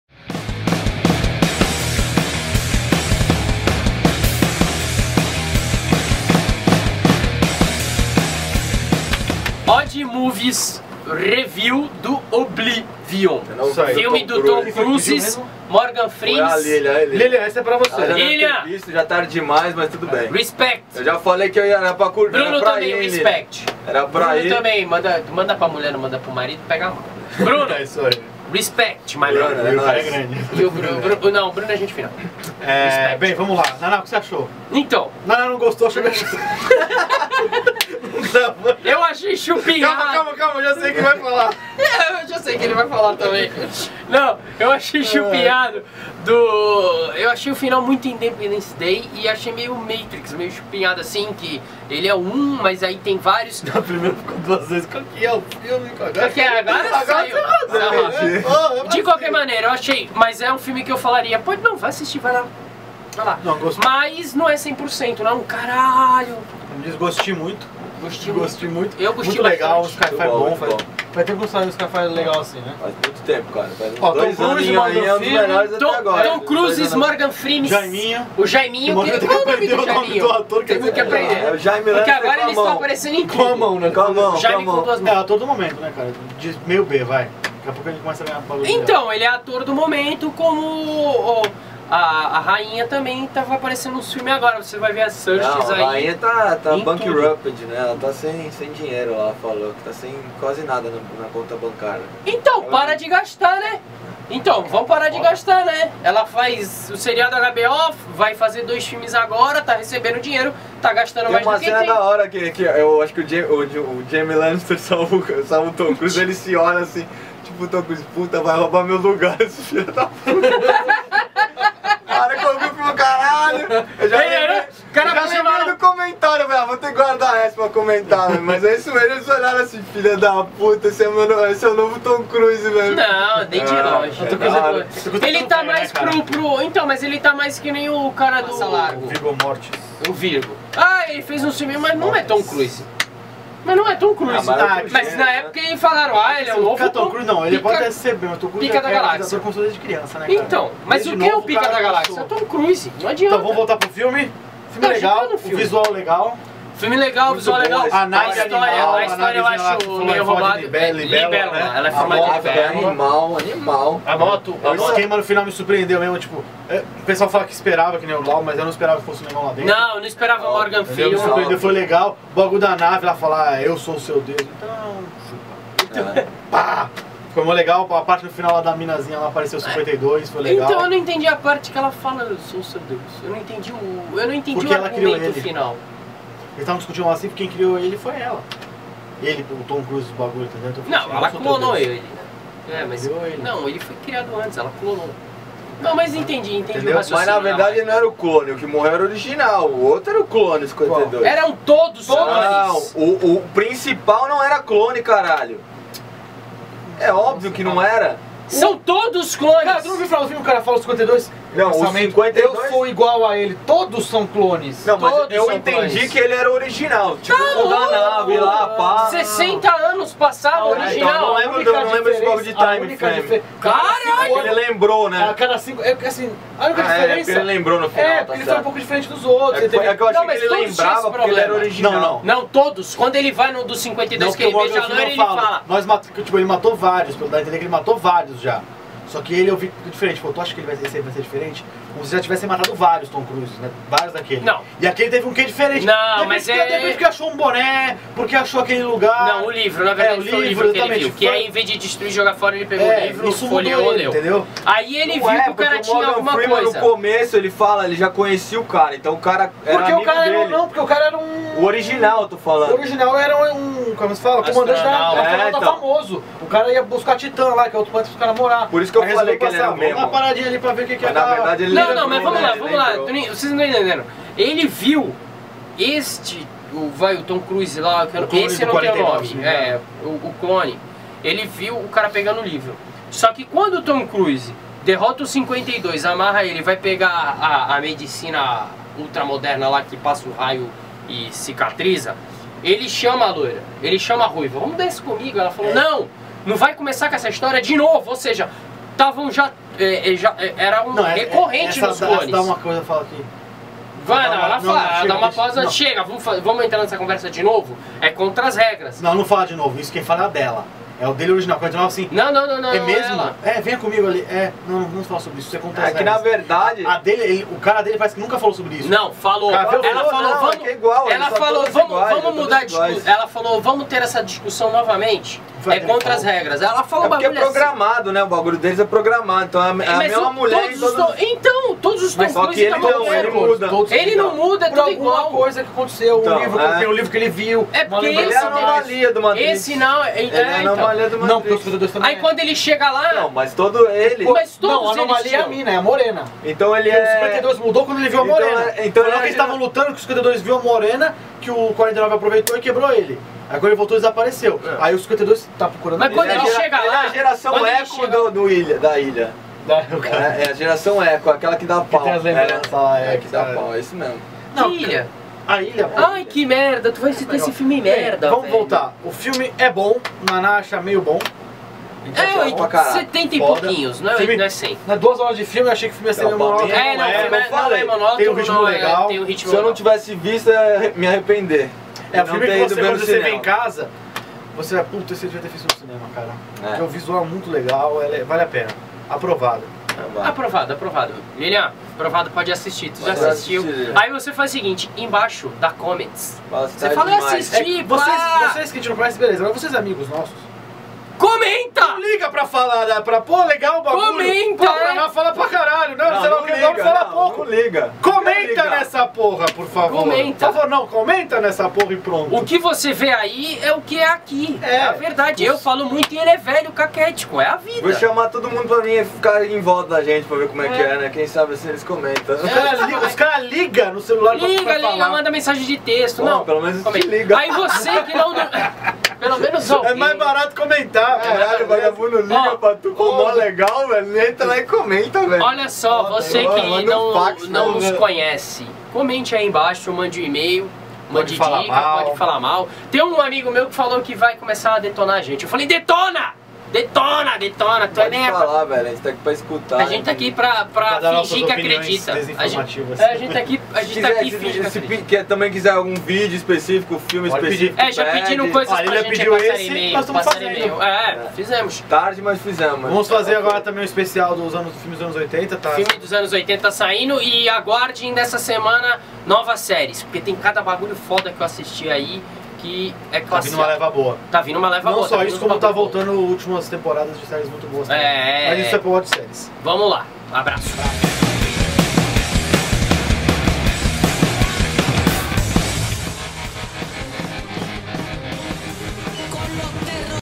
OD Filmes - Oblivion Review por Opiniões Desinformativas
Opiniões Desinformativas apresenta:

OD Filmes - Oblivion Review

REVIEW do filme OBLIVION, estrelado por Tom Cruise, contando ainda com Morgan Freeman.